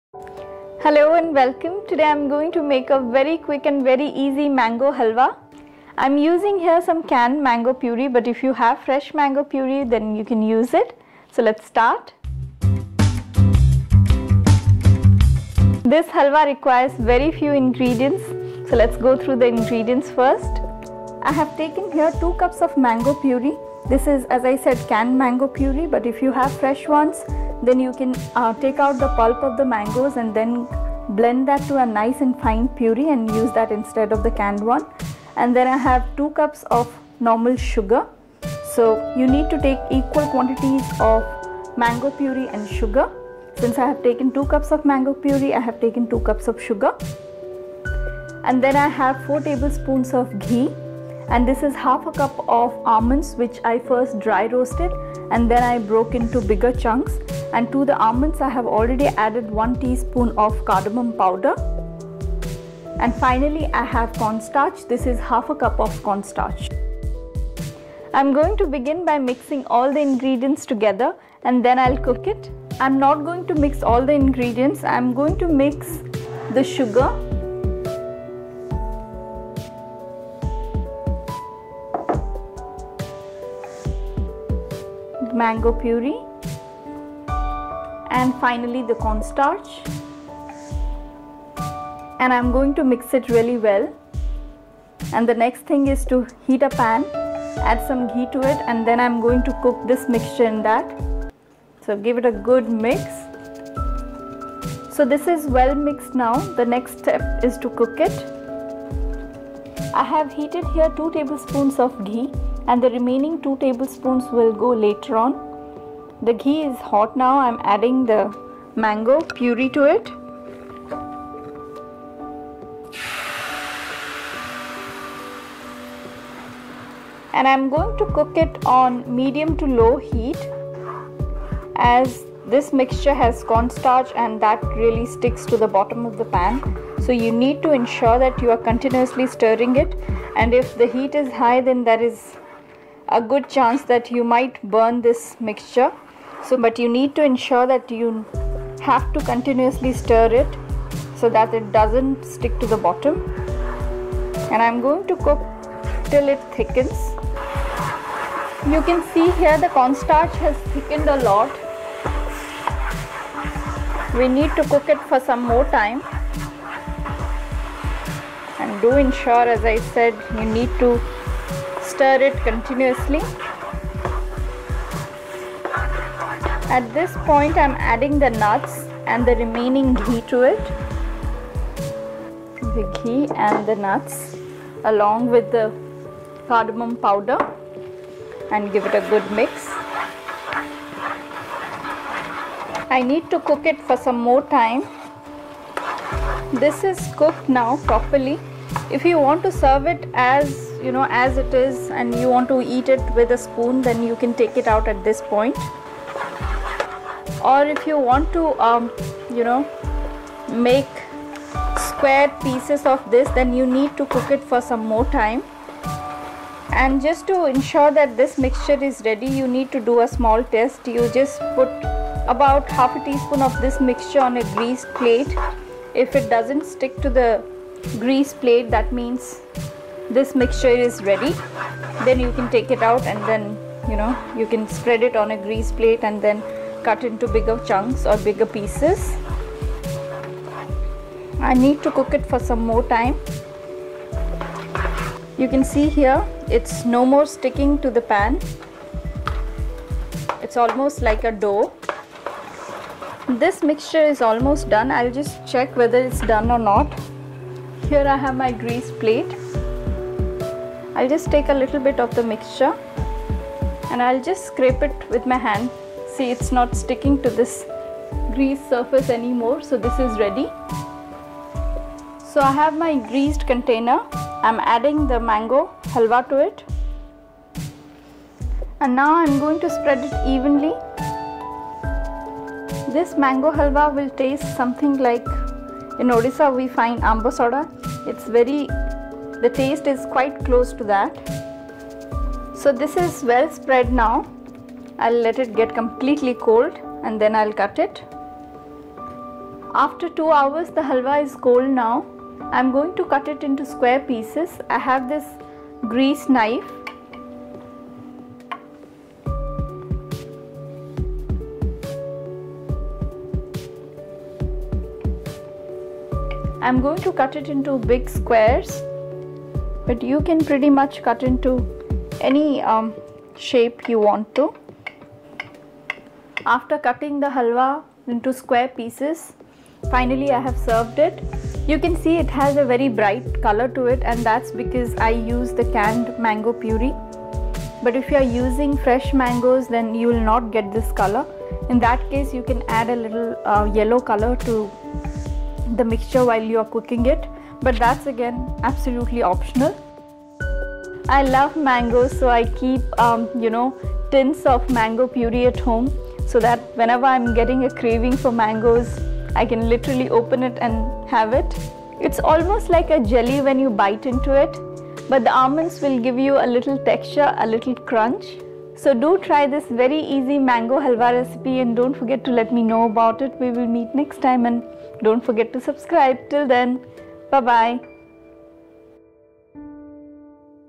Indian Style Mango Halwa Dessert Recipe In English| Instant Canned Mango Pulp Mango Halwa In English
Mango Halwa is made with cooking mango pulp and corn starch together in ghee; Mango Halwa is garnished with nuts ,Thus it is an easy to make mango dessert recipe .So try this very delicious  Mango Halwa recipe and share your comments .

=PLNZR6x68gdTGFFT5IAA8KJyo67vZvvUUF